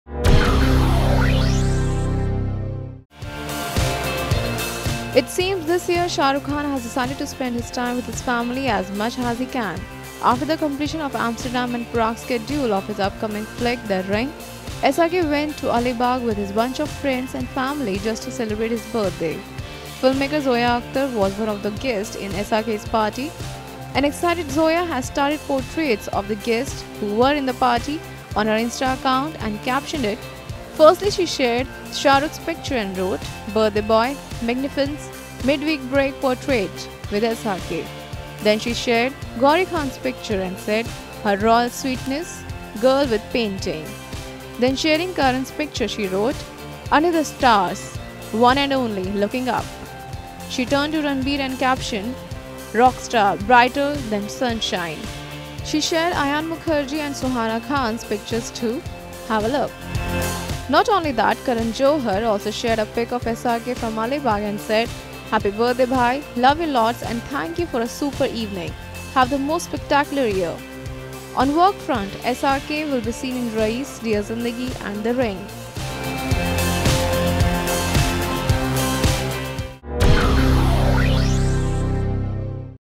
AlibaugDiaries- King Khan birthday with FAMILY & FRIENDS
It seems, this year, Shah Rukh Khan has decided to spend his time with his family as much as he can. After the completion of Amsterdam and Prague schedule of his upcoming flick ‘The Ring’, SRK went to Alibaug with his bunch of friends and family just to celebrate his birthday.